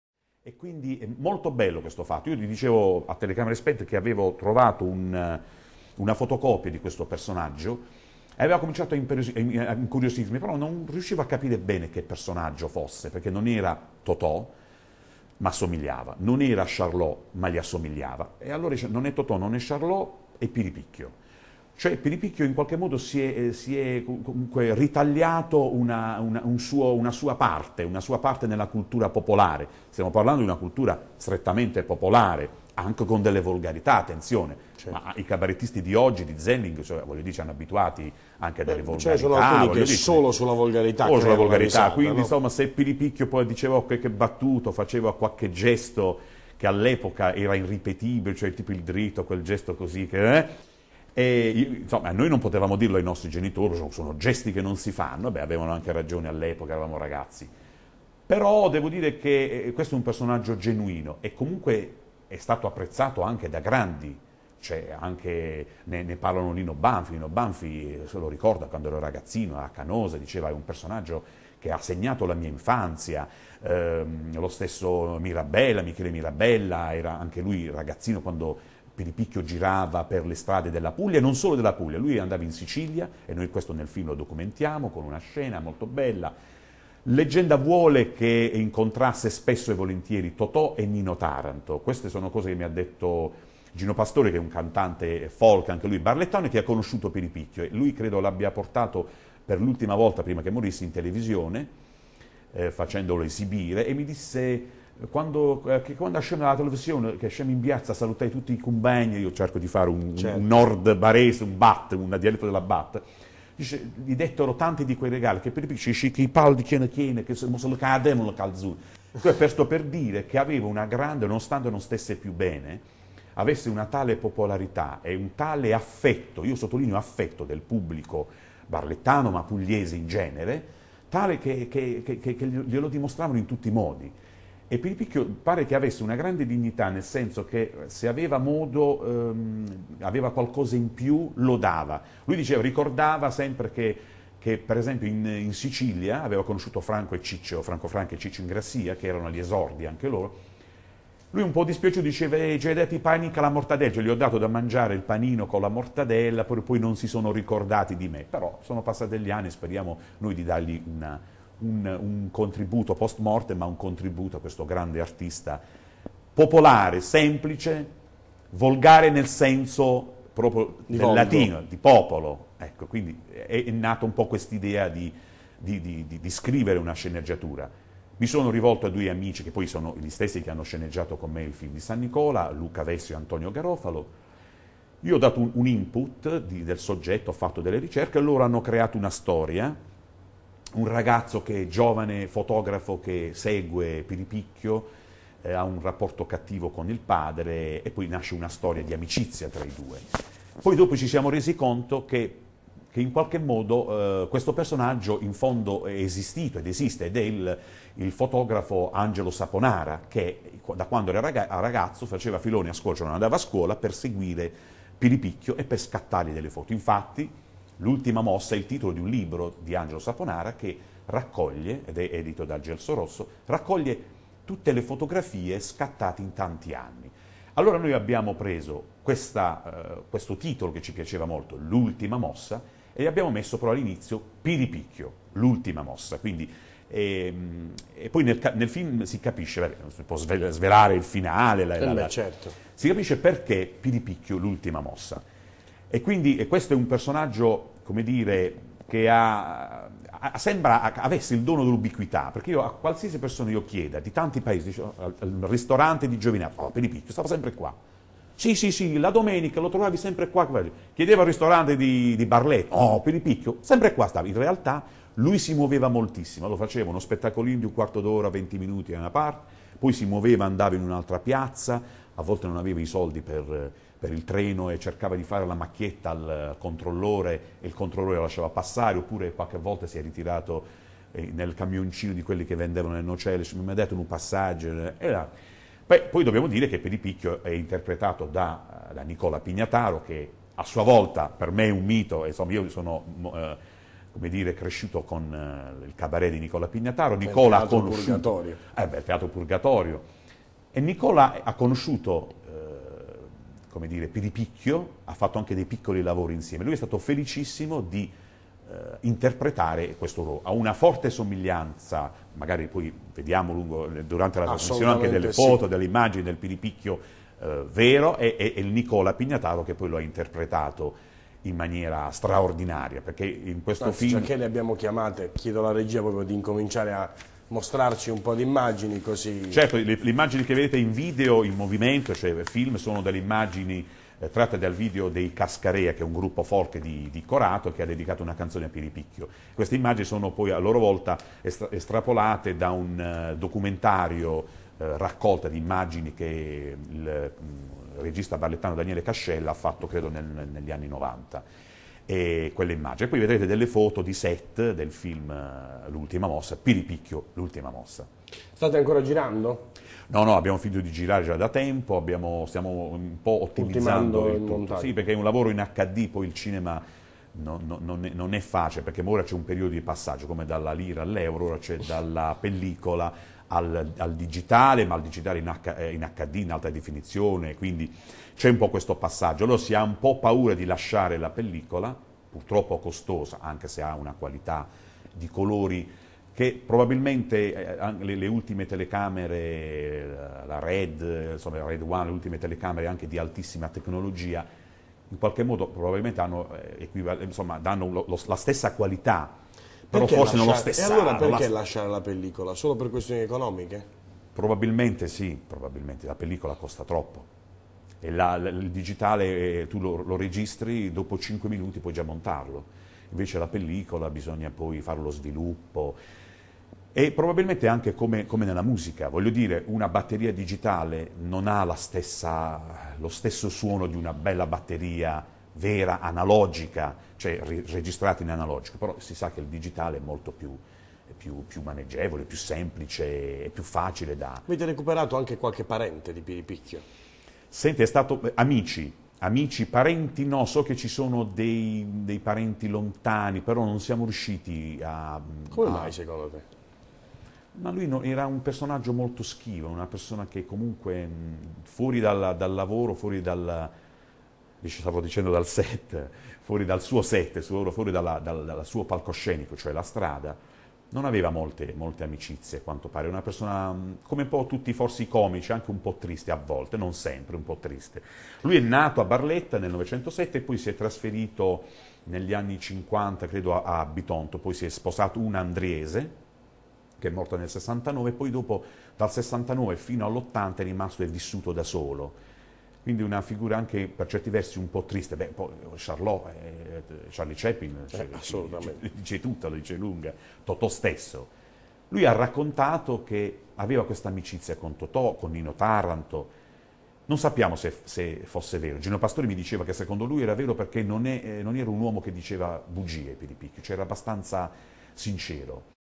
Container intervista a Vito Giuss Potenza
seconda parte dell'intervista al regista Vito Giuss Potenza